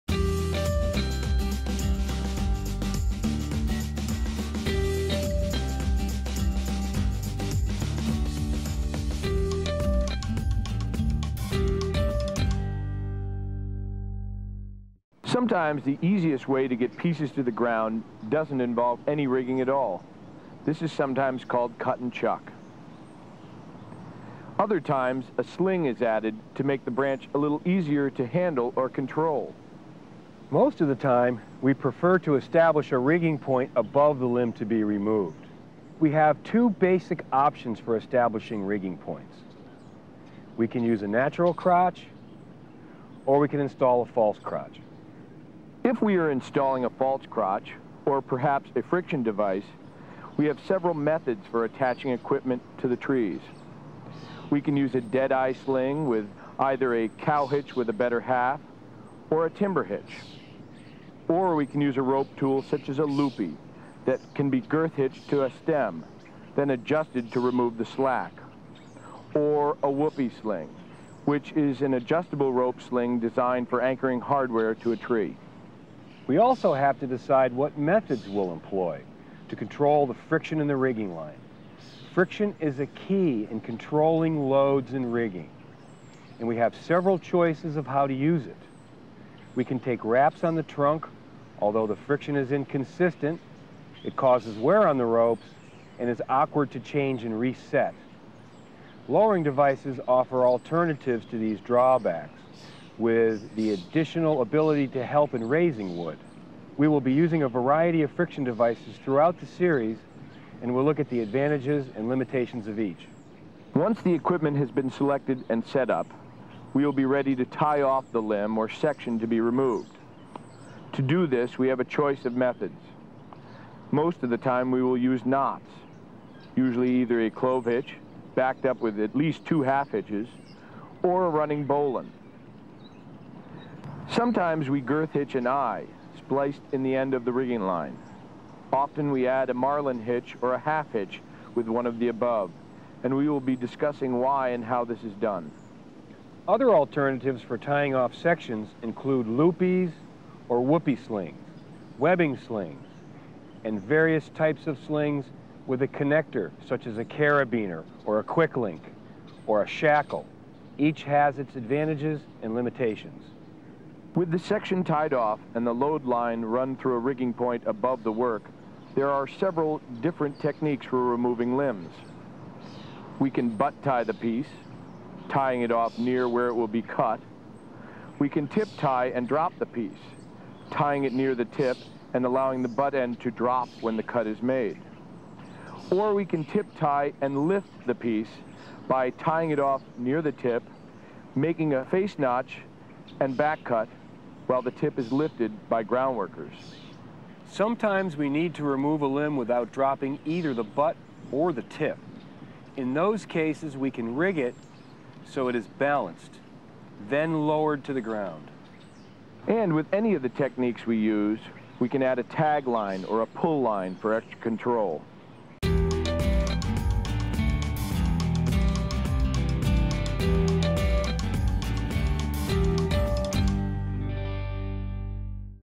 Fundamental Rigging Techniques
Explore the different strategies and equipment used for rigging. This Climbing and Rigging episode is an overview of techniques, equipment, and cuts used in rigging. This video describes different knots that may be used along with each technique.